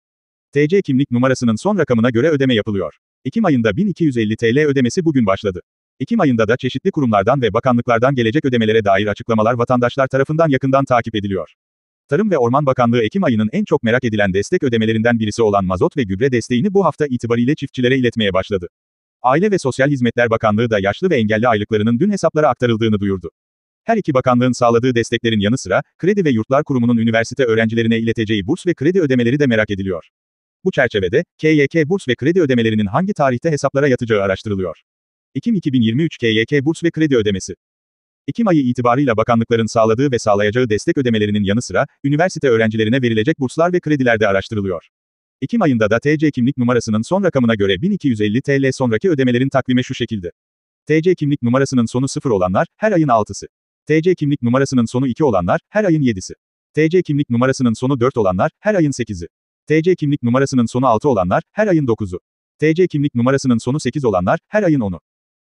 TC Kimlik Numarasının Son Rakamına Göre Ödeme Yapılıyor! Ekim Ayında 1250 TL Ödemesi Bugün Başladı
Ekim ayında da çeşitli kurumlardan ve bakanlıklardan gelecek ödemelere dair açıklamalar vatandaşlar tarafından yakından takip ediliyor.
Tarım ve Orman Bakanlığı ekim ayının en çok merak edilen destek ödemelerinden birisi olan mazot ve gübre desteğini bu hafta itibariyle çiftçilere iletmeye başladı. Aile ve Sosyal Hizmetler Bakanlığı da yaşlı ve engelli aylıklarının dün hesaplara aktarıldığını duyurdu. Her iki bakanlığın sağladığı desteklerin yanı sıra, Kredi ve Yurtlar Kurumu'nun üniversite öğrencilerine ileteceği burs ve kredi ödemeleri de merak ediliyor. Bu çerçevede, KYK burs ve kredi ödemelerinin hangi tarihte hesaplara yatacağı araştırılıyor.
Ekim 2023 KYK Burs ve Kredi Ödemesi
Ekim ayı itibarıyla bakanlıkların sağladığı ve sağlayacağı destek ödemelerinin yanı sıra, üniversite öğrencilerine verilecek burslar ve krediler de araştırılıyor. Ekim ayında da TC kimlik numarasının son rakamına göre 1250 TL sonraki ödemelerin takvime şu şekilde:

TC Kimlik Numarasının Sonu 0 Olanlar: Her ayın 6’si
TC Kimlik Numarasının Sonu 2 Olanlar: Her ayın 7’si
TC Kimlik Numarasının Sonu 4 Olanlar: Her ayın 8’i
TC Kimlik Numarasının Sonu 6 Olanlar: Her ayın 9’u
TC Kimlik Numarasının Sonu 8 Olanlar: Her ayın 10’u #2023